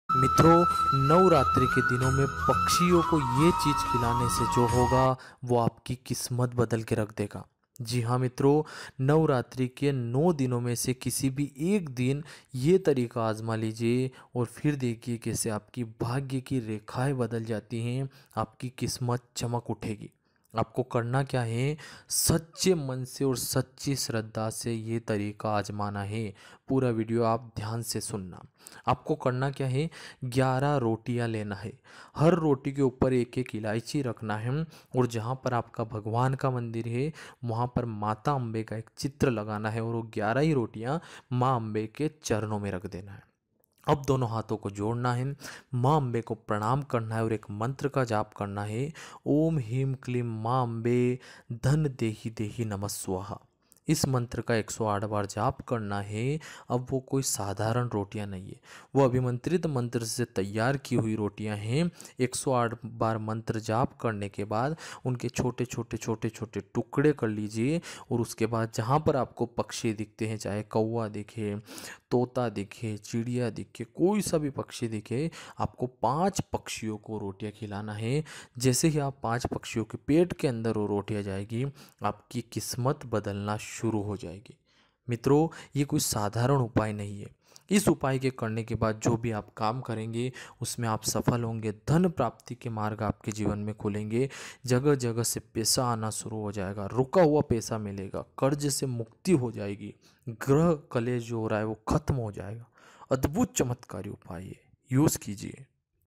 नवरात्री में पक्षियों को ये चीज खिला दो फिर क्या होगा ! navratri me pakshiyo ko khilaye !
नवरात्री में पक्षियों को ये चीज खिलाने से आपका भाग्य चमक उठेगा !
आपका हर काम सफल होगा